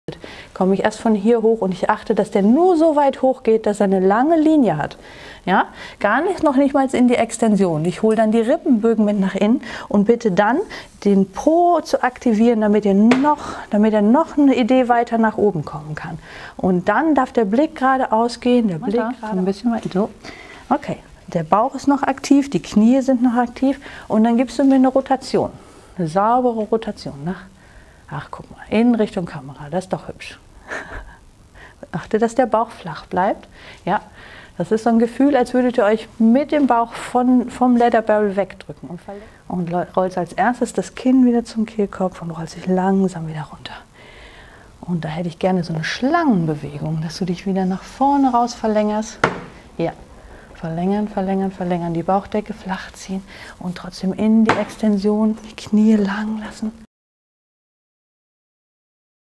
Anleitungen und Variationen für die Wirbelsäulenextension mit Antje Korte
Dieses Tutorial zeigt Euch Variationen und Korrekturmöglichkeiten, um Wirbelsäulenextensionen am Ladder Barrel effektiver und präziser zu gestalten.
Level: alle — Trainer: Antje Korte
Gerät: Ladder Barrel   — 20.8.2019
Dauer: 9,5 Minuten